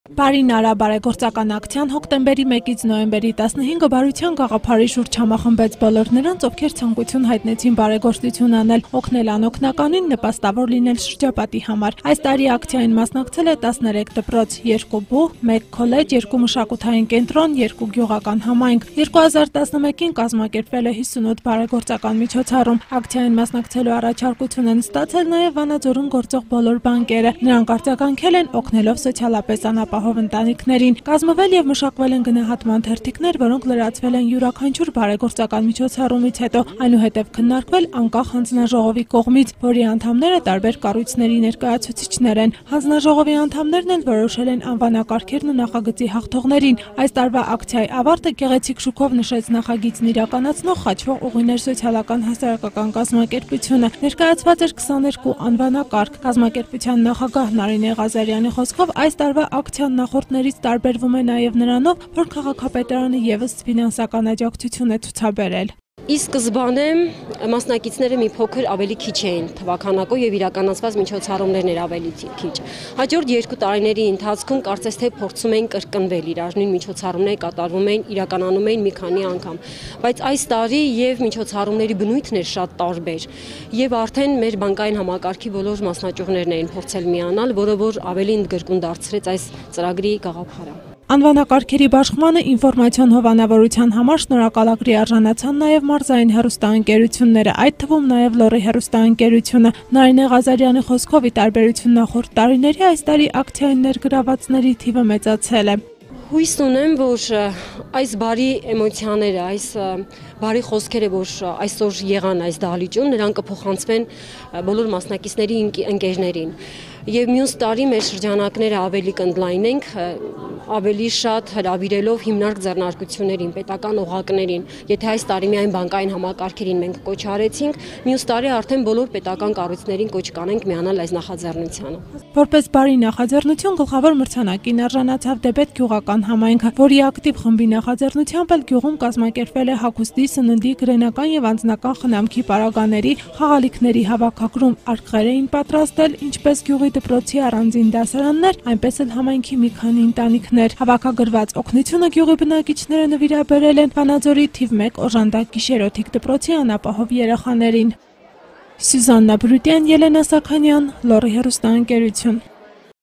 «Բարին արա» ակցիան մոտեցավ ավարտին
«Խաչվող ուղիներ» ՍՀԿ-ի կողմից իրականացվող «Բարին արա» ակցիան մոտեցավ իր ավարտին, ինչի կապակցությամբ կազմակերպվել էր գեղեցիկ միջոցառում, որին պարգեւատրվեցին 22 անվանակագերում ներկայացված դպրոցները, ԲՈՒՀ-երն ու գյուղական համայնքները: Ամենաբարի նախաձեռնության համար պարգեւատրվեց Դեբեդ գյուղական համայնքը: Հատուկ մրցանակի արժանացավ նաեւ նախագծի ինֆորմացիոն համահովանավոր «Լոռի» հեռուստաընկերությունը: